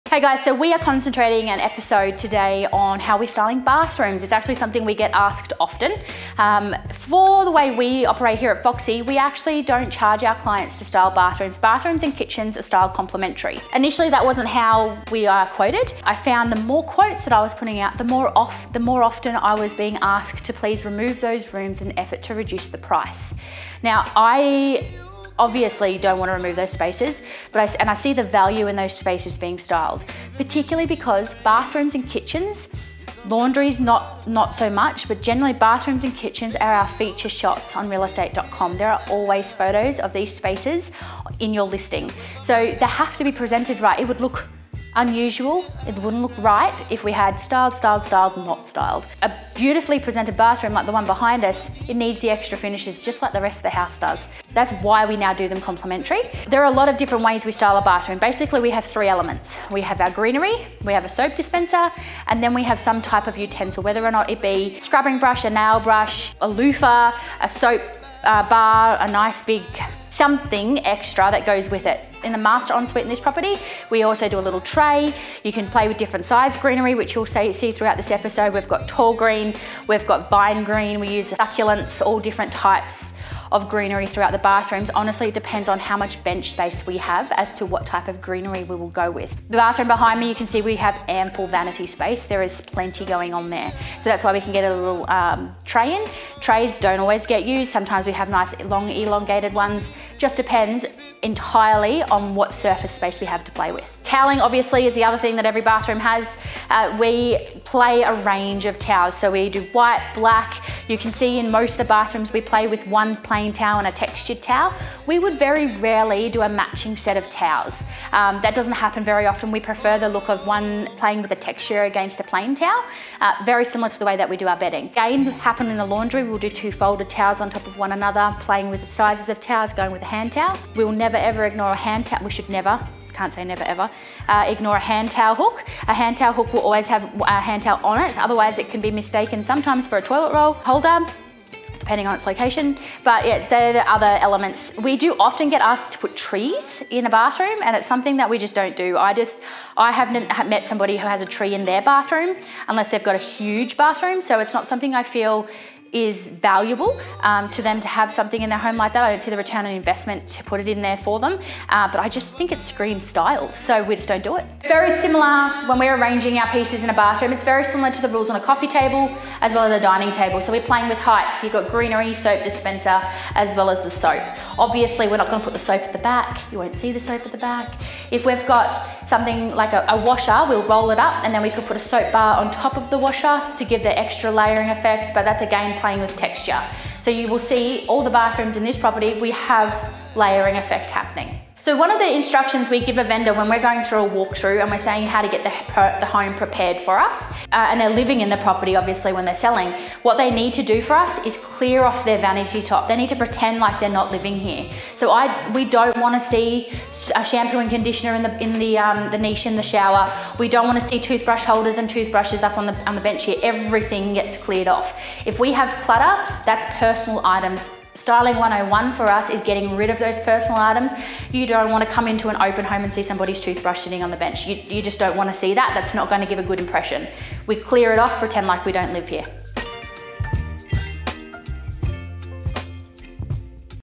How to Style Bathrooms (When Staging to Sell)
In episode 196 of Foxy TV Phoebe shows us how we style bathrooms when staging to sell. She touches on decluttering, accessory formation, towels and more.